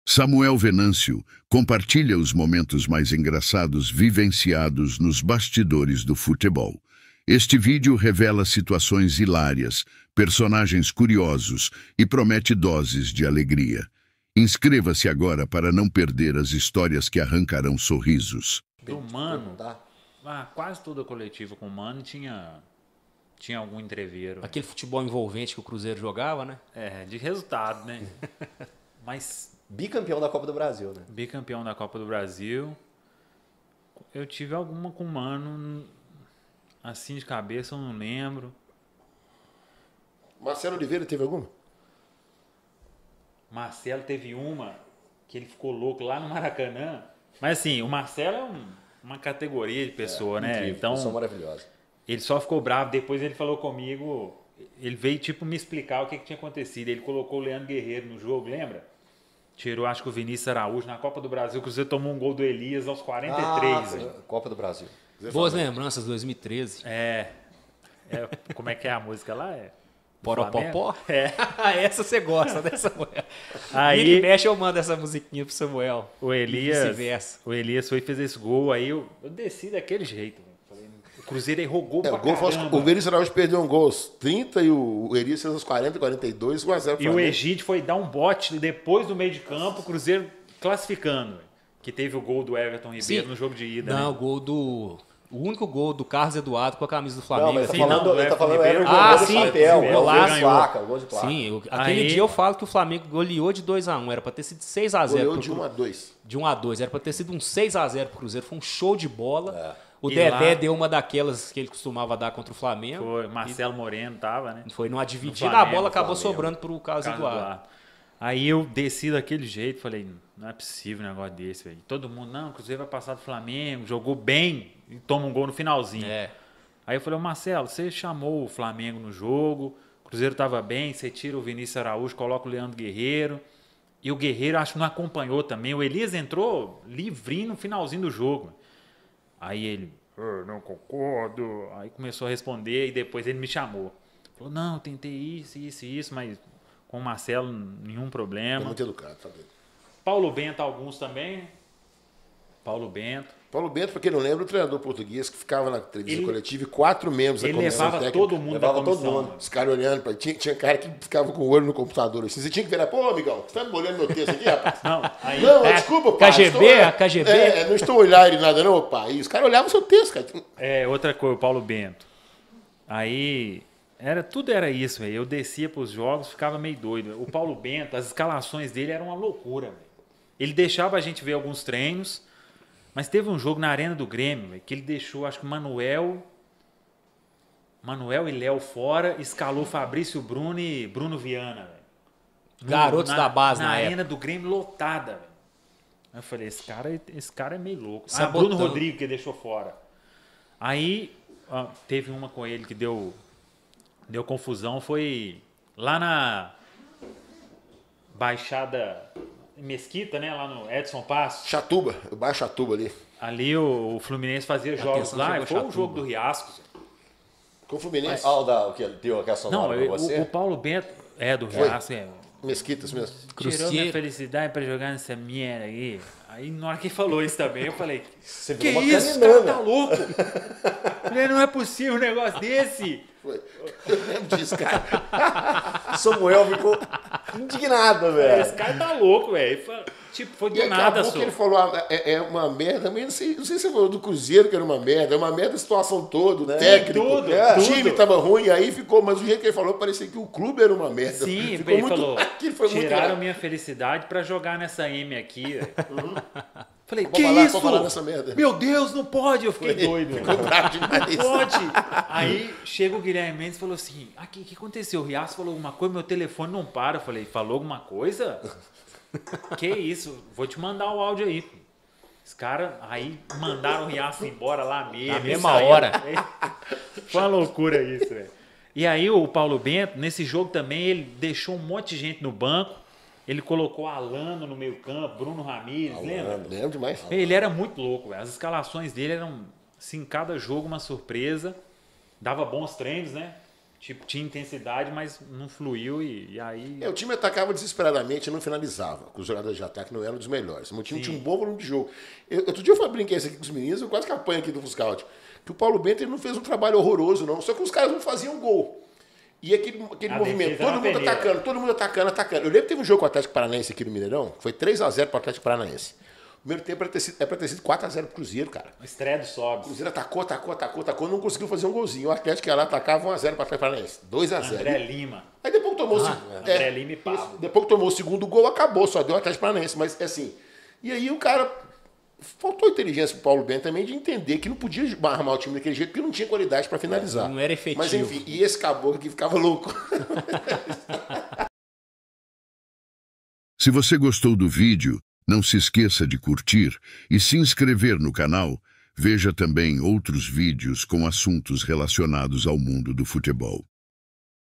RESENHA DE SAMUEL VENÂNCIO E MARCELO OLIVEIRA
Samuel Venâncio Diverte: Risos nos Bastidores do Futebol! 😄⚽

Prepare-se para descontrair com Samuel Venâncio enquanto ele compartilha os momentos mais hilários que vivenciou nos bastidores do futebol. Este vídeo é sua entrada para o lado divertido do esporte, onde risadas e histórias inusitadas se tornam protagonistas.

😂Risos nos Campos e Fora deles: Samuel Venâncio revela situações engraçadas que ocorreram não apenas durante as partidas, mas também nos momentos de descontração fora dos campos. Uma jornada repleta de episódios que arrancaram sorrisos.

🎭 Personagens Cômicos do Futebol: Conheça os personagens curiosos e situações inusitadas que fazem do futebol um terreno fértil para risadas. Samuel destaca os bastidores que muitas vezes passam despercebidos, mas que adicionam doses de humor ao esporte.

Junte-se a Samuel Venâncio e embarque nessa viagem humorística pelos momentos mais engraçados do futebol. 🌟⚽